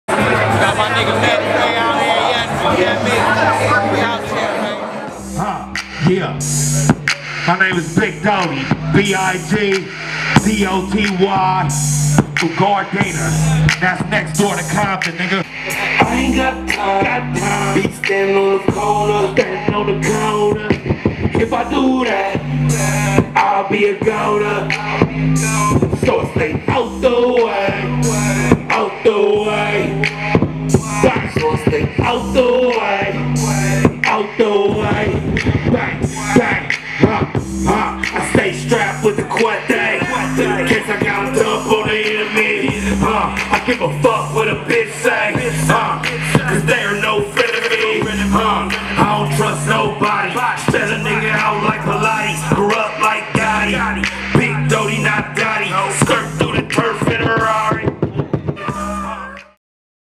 DABnCARTS w/ BIG DOTY
Big Doty Performance at Dab n Carts in Phoenix 10/06/16